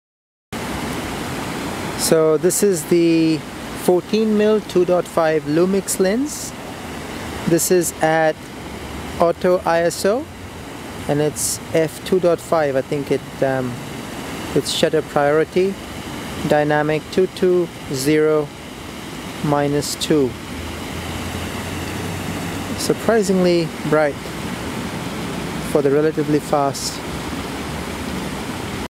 Comparison of wide lenses on GHx - Lumix 14mm f2.5
Comparison of various wide lenses on GH1 with
a. 100Mbps Max Latitude Native 24/25p Patch
b. Dynamic 2 2 0 -2 settings
c. f2 on all lenses to compare with the Olympus 12mm f2
d. Auto ISO
e. 1/50th shutter